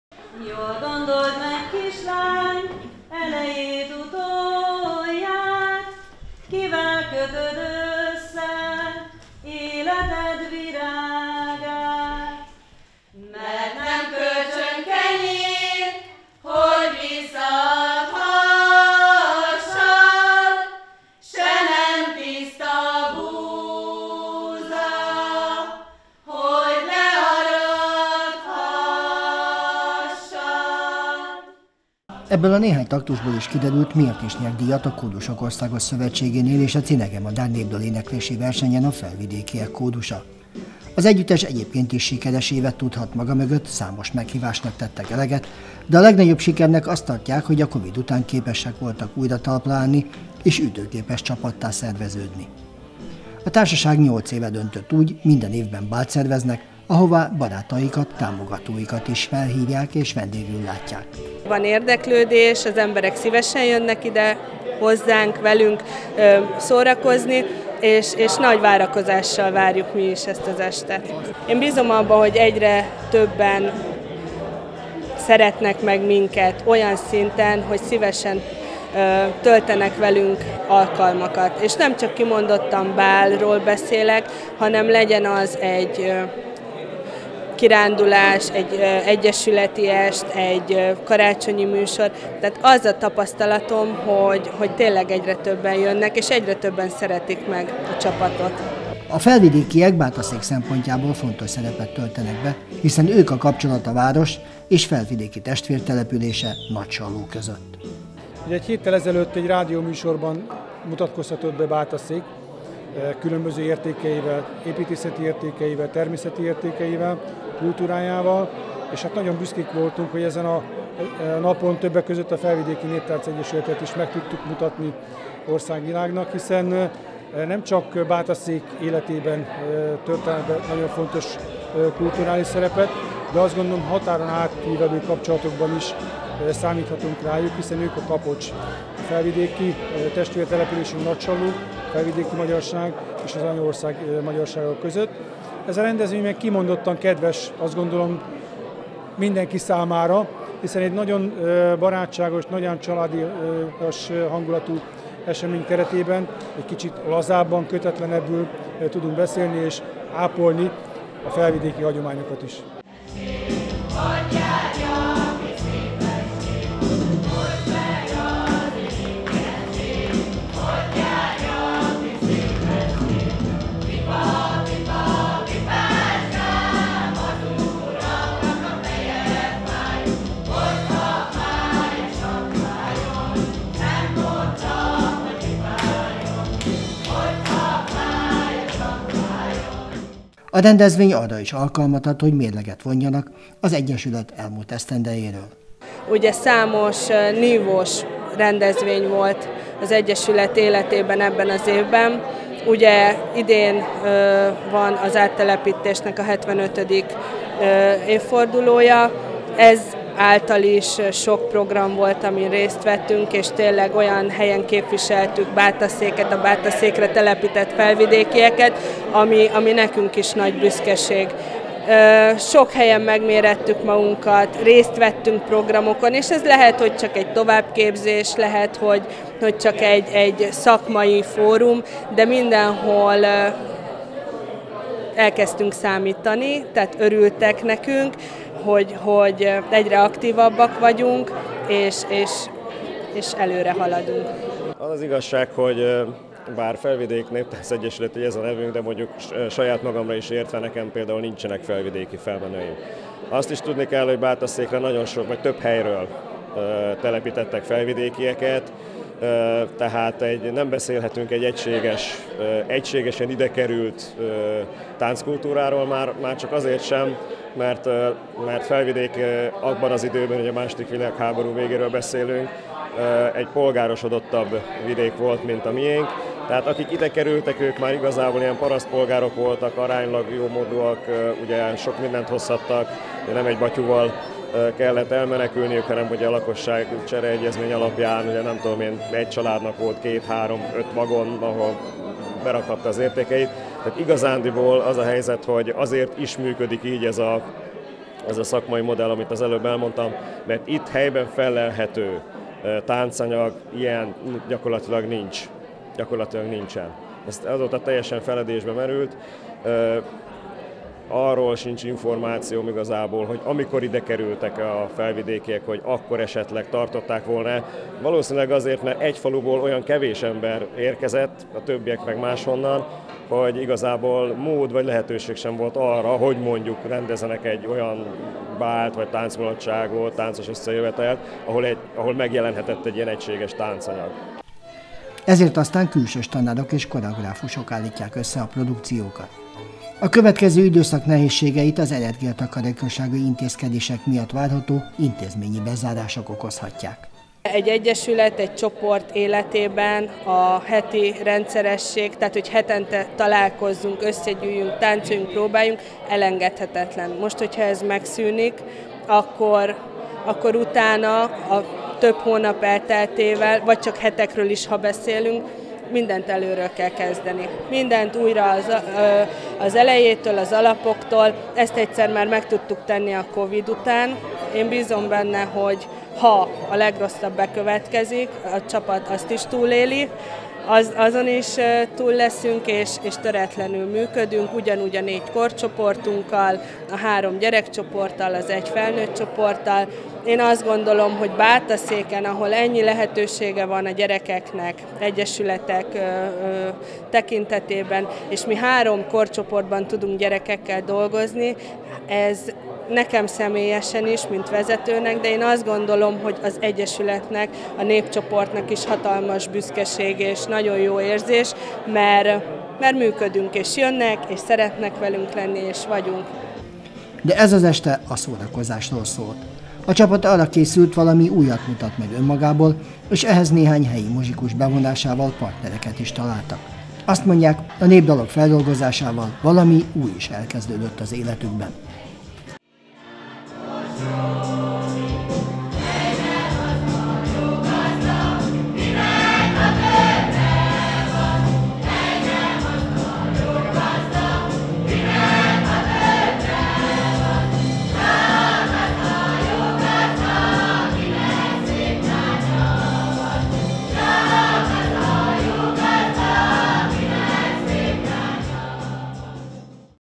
Felvidékiek bálja - Bátaszék
Nyolcadik alkalommal rendezett bált a Felvidéki Néptánc Egyesület. A szervezet társadalmi elismertségét igazolja, hogy folyamatosan nő a csoport mögötti támogatói kör. Valamint jelentős szakmai sikerként könyvelik el, hogy a csoport lányaiból megalakult énekkar arany minősítést szerzett a Cinegemadár Népdaléneklési versenyen.